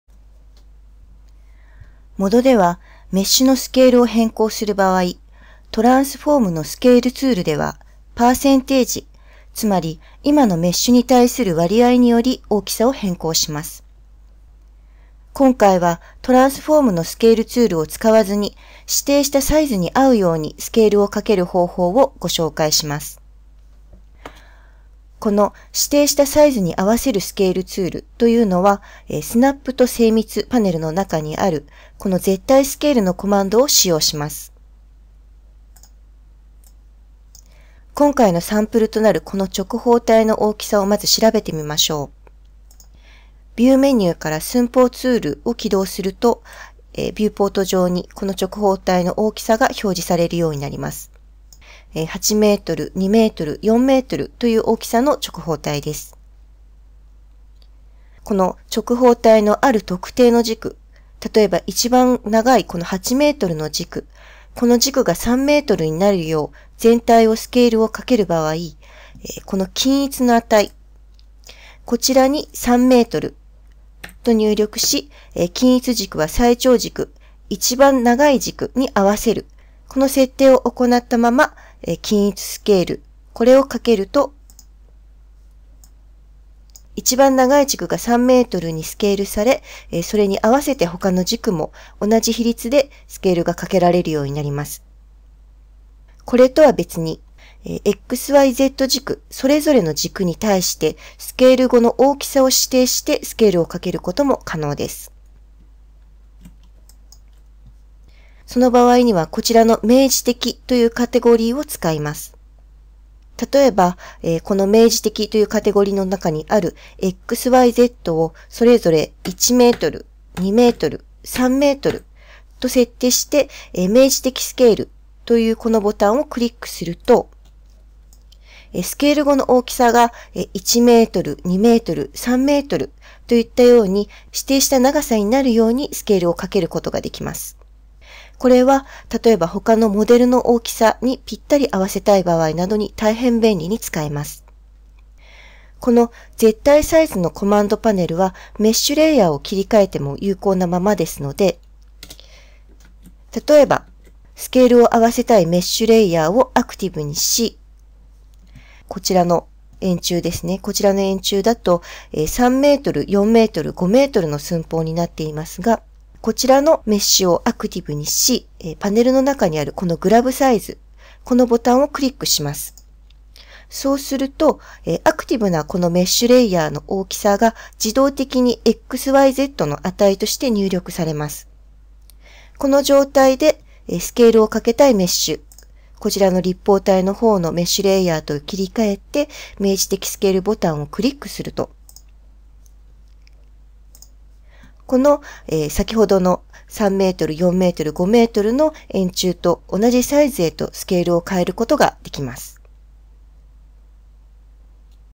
【modo機能紹介】大きさに合わせてスケールを行うには
トランスフォームのスケールツールを使わず、指定したサイズに合わせるようスケールをかける方法をご紹介します。

その他の機能紹介・チップスについては、こちらのサイトをご覧ください：